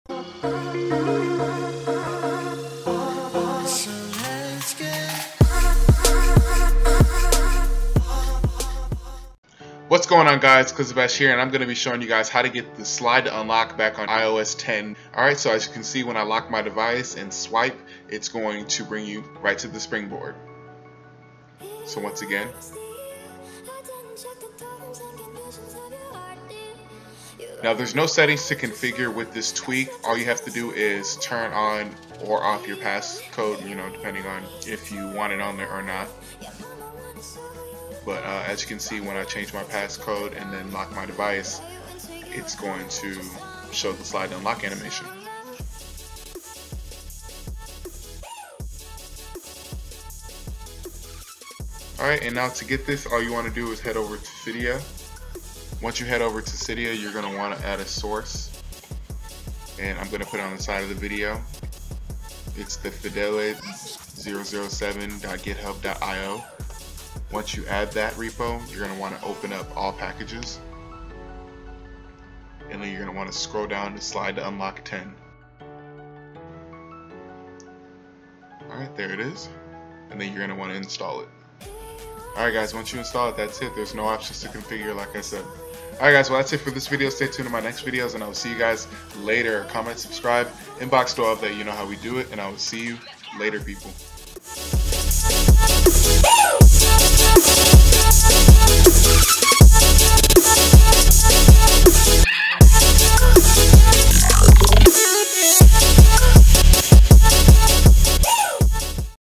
How to get Slide To Unlock Back on IOS 10-10.2!
SLIDE TO UNLOCK IS BACK!
Repo: fidele007.github.io